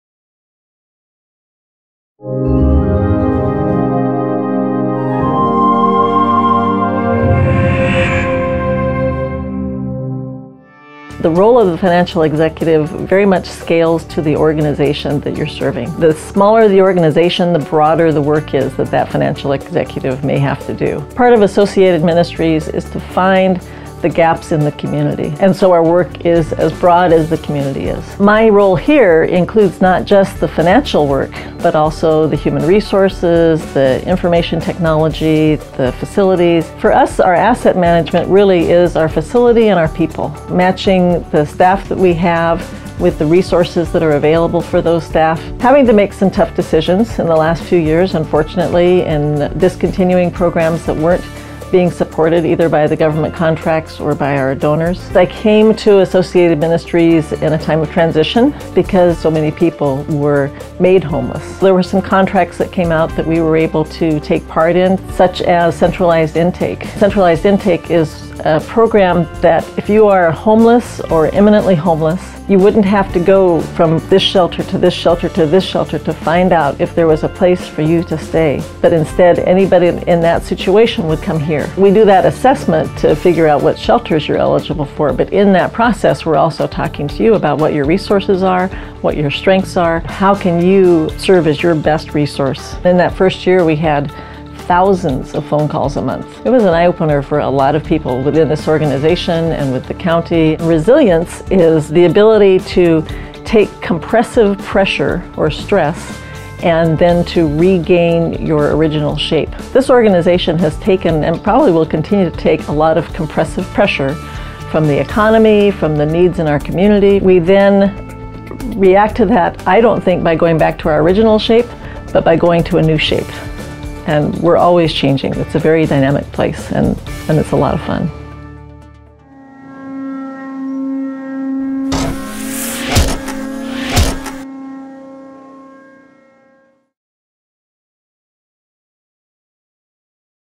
Financial Executive of the Year: Molly Archer
In May of 2013, A.C.T. Video Productions had the honor of interviewing the winners of the Business Examiner's prestigious "Financial Executives of the Year" award.  Our profile videos introduced each honoree during the awards ceremony at Historic 1625 Tacoma Place.